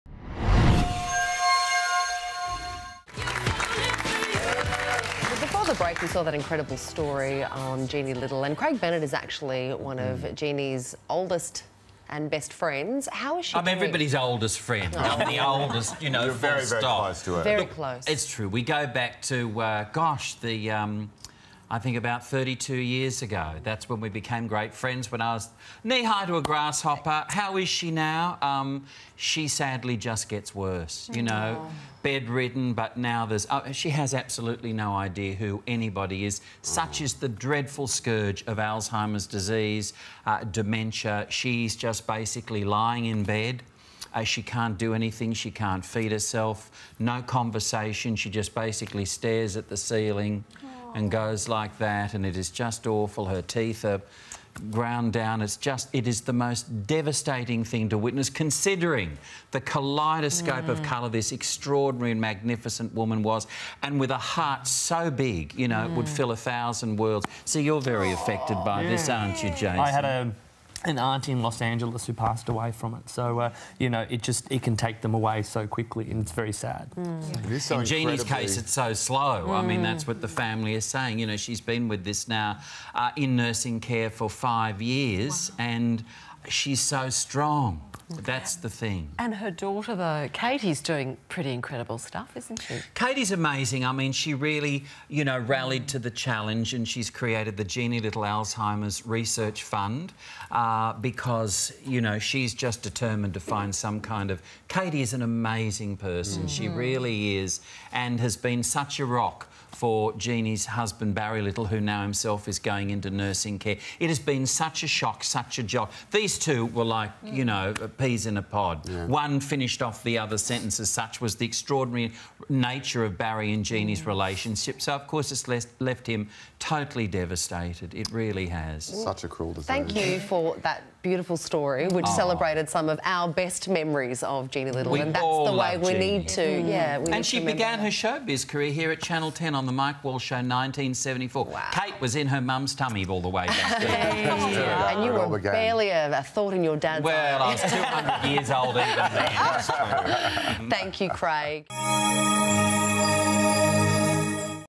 Jeanne Little Update
Her best friend of 32 years, Craig Bennett, has an update on Jeanne Little's situation.